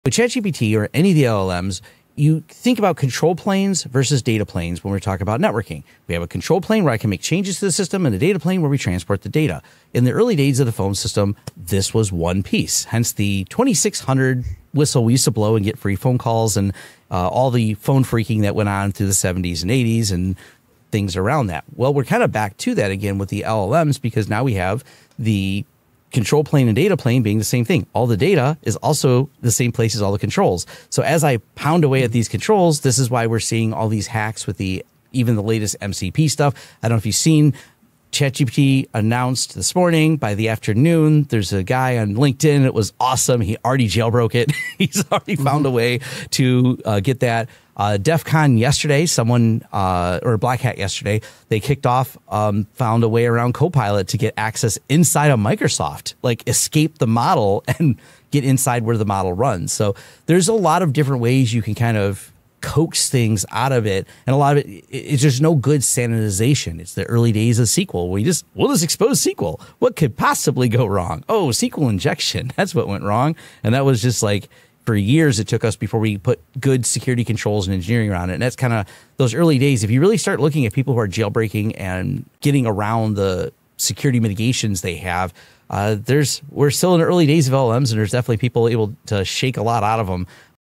LLM Security: Hacking ChatGPT & the Control Plane
LLMS: a double-edged sword? Data &
controls are now one. Experts see echoes of
early SQL days-'What could possibly go
wrong?'